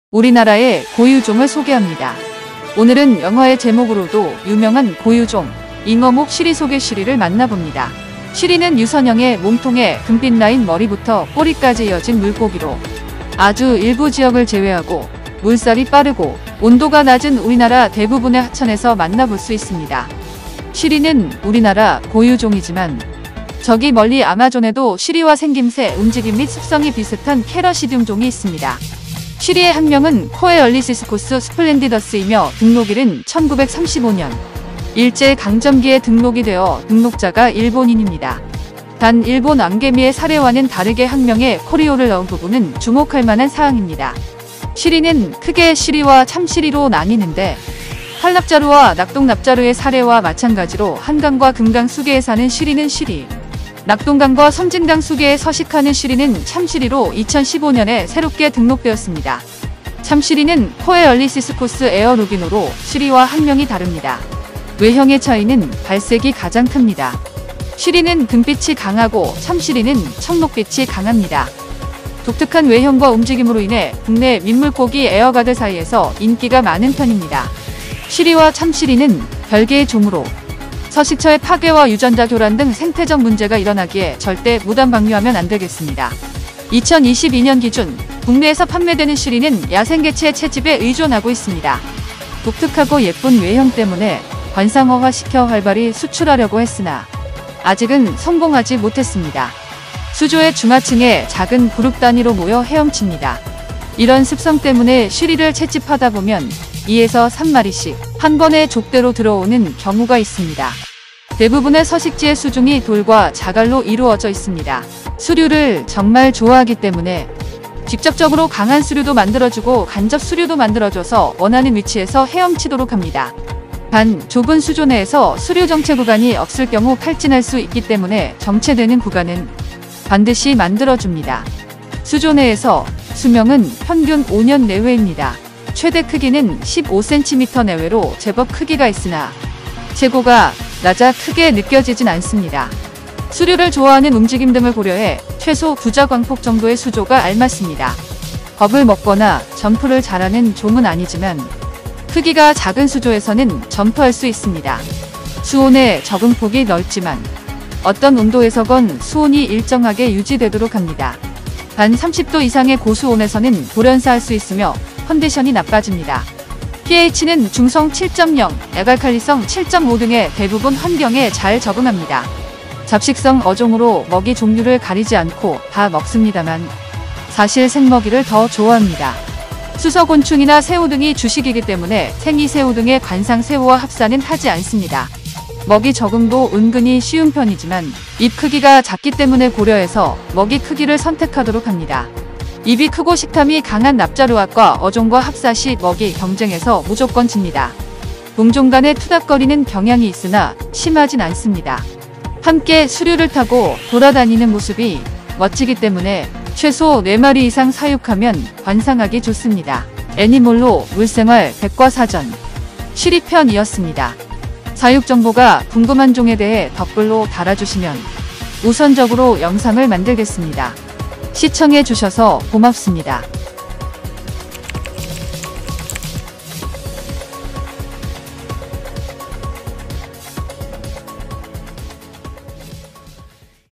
쉬리, 집 어항에서 키울수 있을까? [물생활백과사전 ]
물생활 백과사전 쉬리를 한 번 키워봅니다.
쉬리와 참쉬리의 차이점도 알아보고,
사육정보도 알아볼까요?

다큐 물생활 전문채널.

물생활 관련 문의사항은 저희 네이버 카페에 남겨주세요!